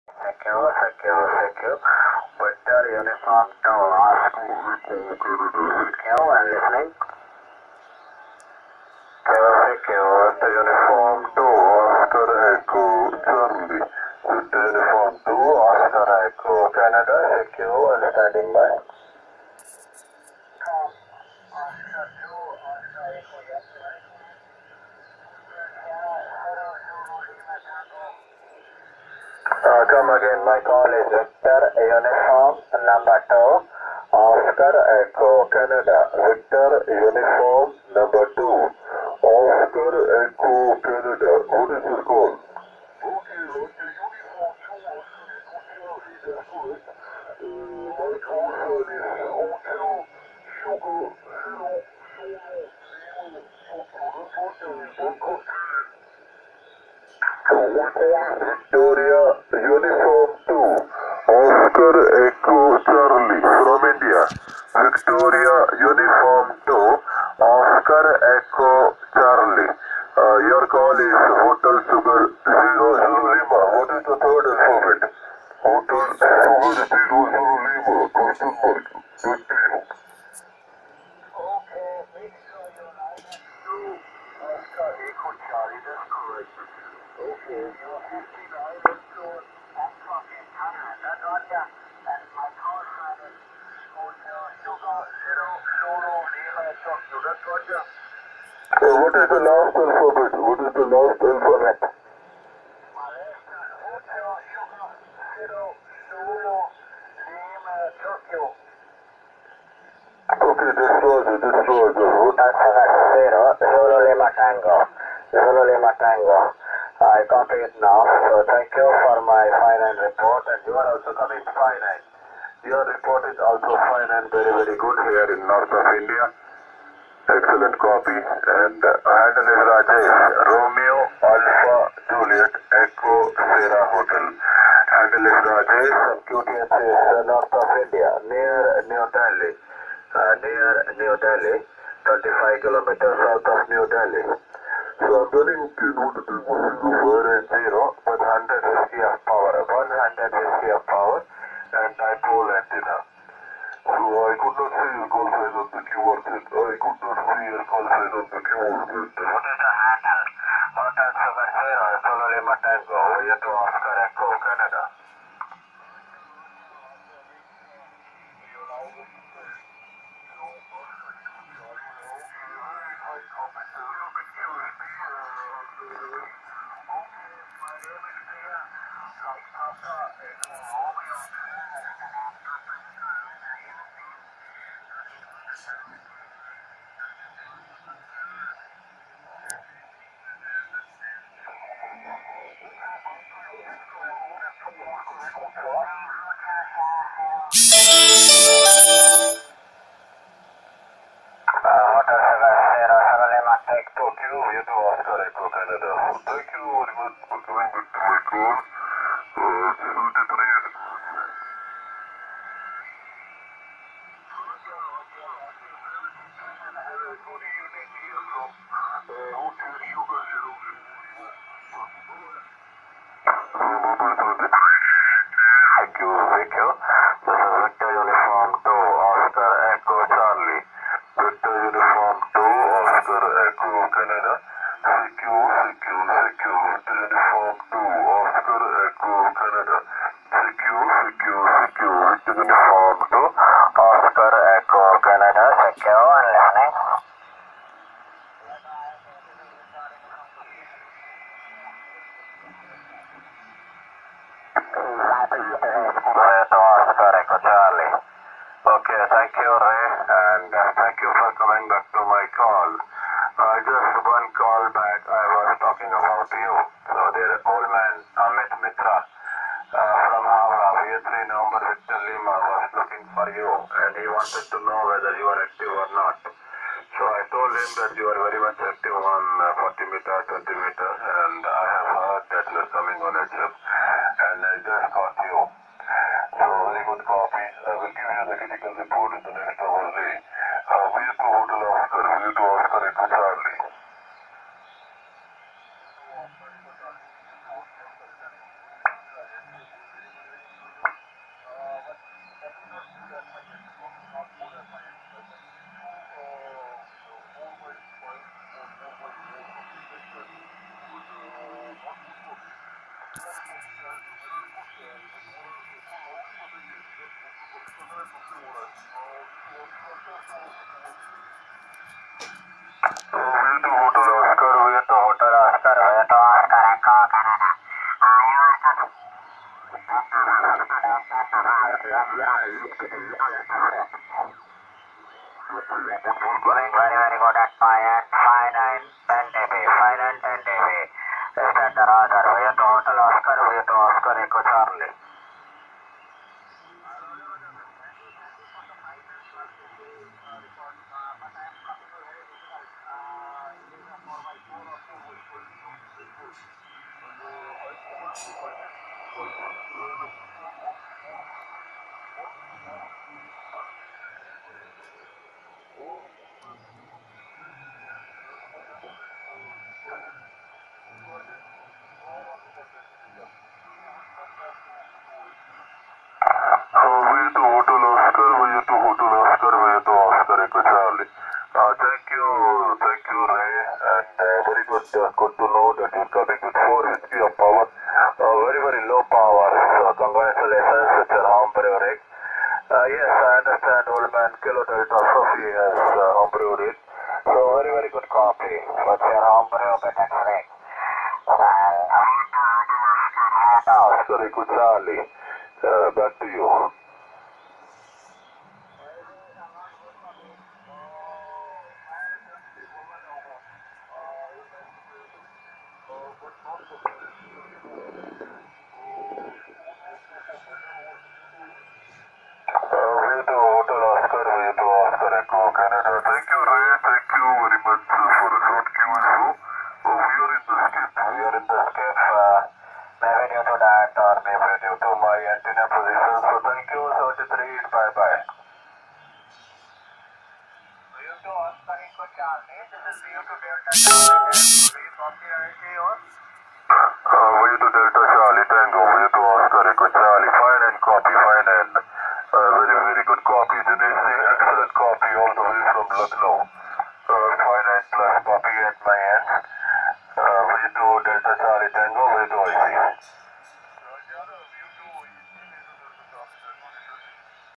VU2UUU listening to 20m QSO with homebrew BITX20 transceiver.
Trying to setup the Bitx20 Ham Radio Transceiver.